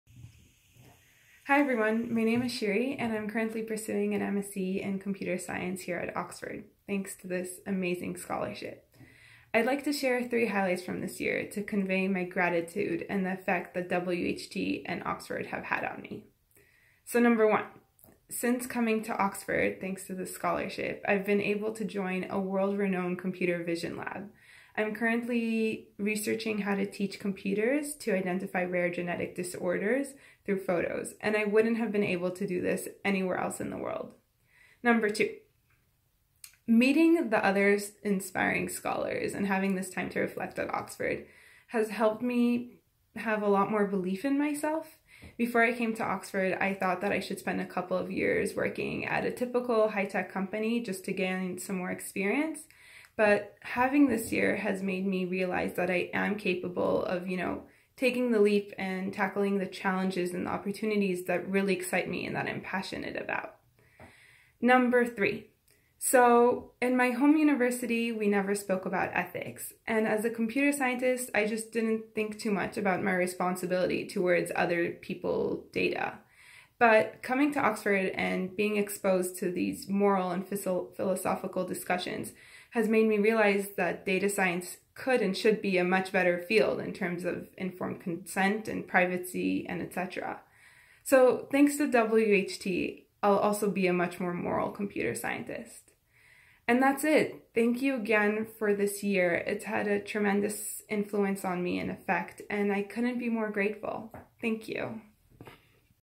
WHT Scholar Shiri Avni - 3 highlights of Leadership Programme & coming to Oxford
MSc Computer Science Scholar,  Shiri Avni from Israel on what the Weidenfeld-Hoffmann Scholarship & Leadership Programme and graduate study at the University of Oxford has meant to her.